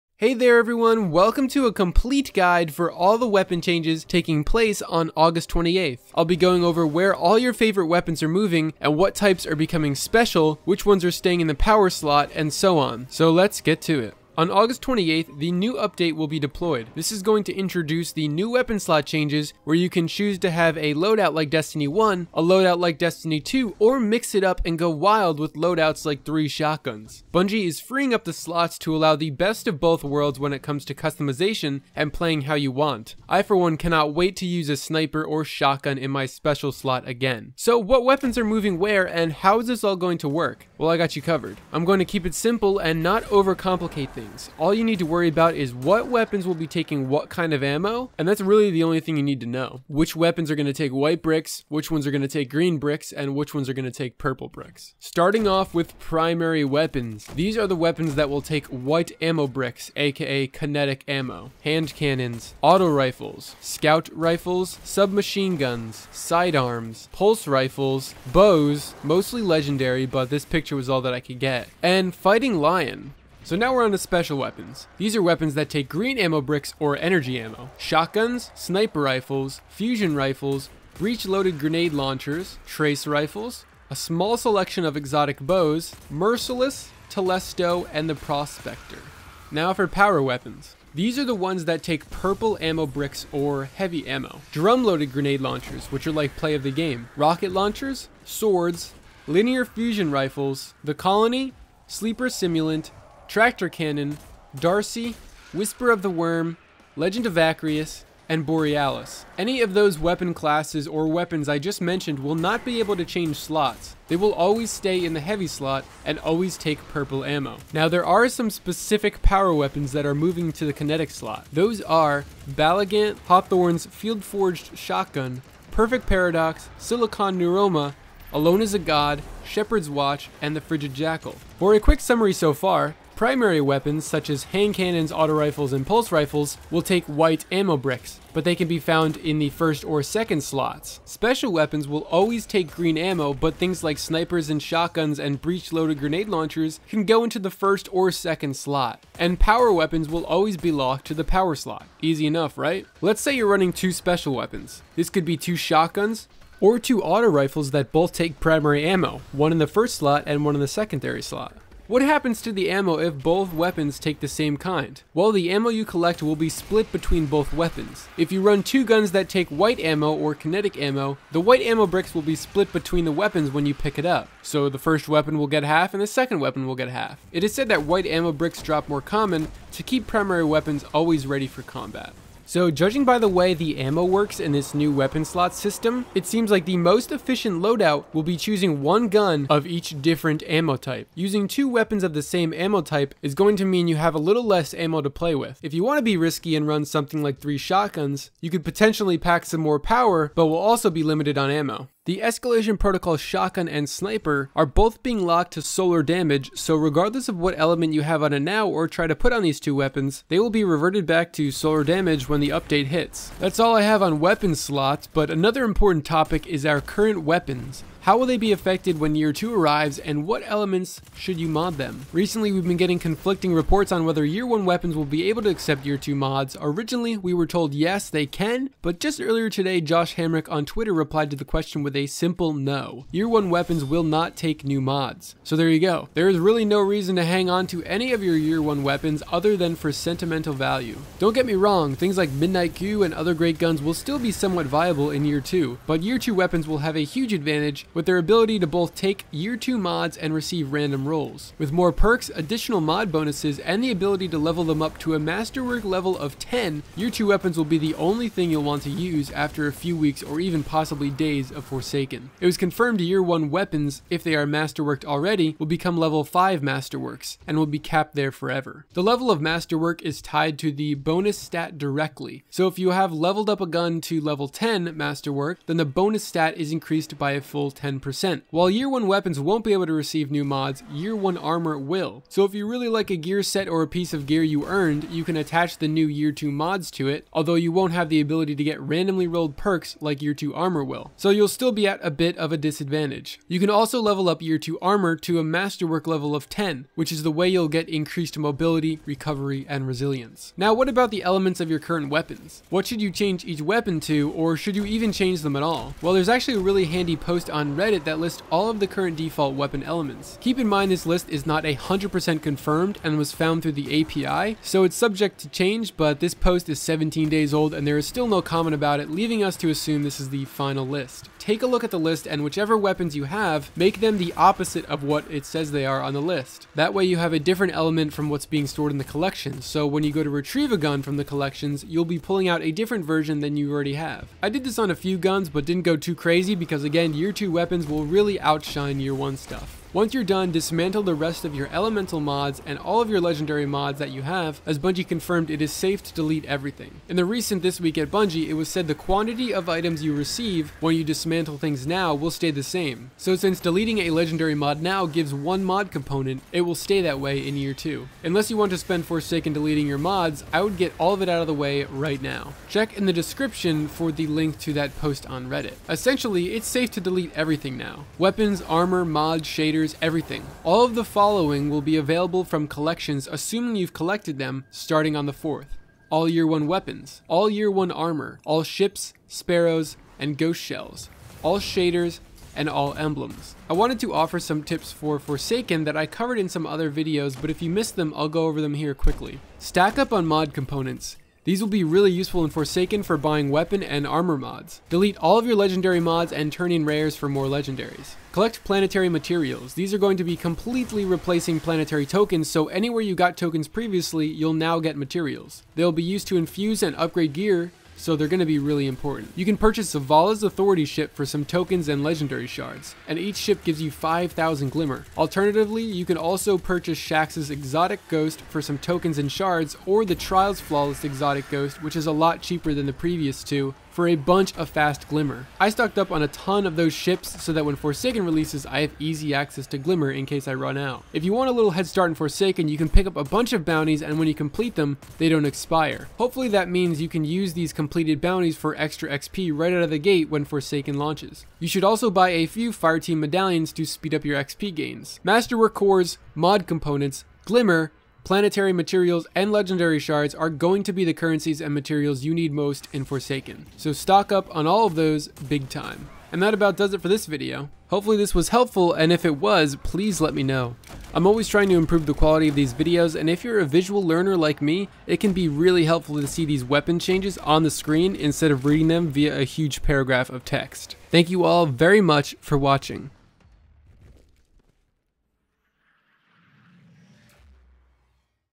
Everything We Know About Weapon Slots, Mods, and Gear Changes in Destiny 2! (What to Save or Hoard)
In this video I cover how the new Weapon Slot changes will work and impact the game, how Year 1 weapons and armor will be affected, and what is safe to delete vs what you should be hoarding. I also provide some tips for being prepared for Forsaken. Weapon Element Reddit post

Things that are safe to delete:
- Armor
- Weapons
- Mods
- Ships, Ghost Shells, and Sparrows
- Shaders
- Ikora Rey Tokens

Things that are up in the air:
- Trials Tokens
- Calus Tokens
- Faction Tokens

Things you should hoard:
- Vanguard and Crucible Tokens / Gunsmith Parts
- Planetary Materials (keep tokens for now)
- Legendary Shards
- Mod Components
- Quick sources of Glimmer
- Masterwork Cores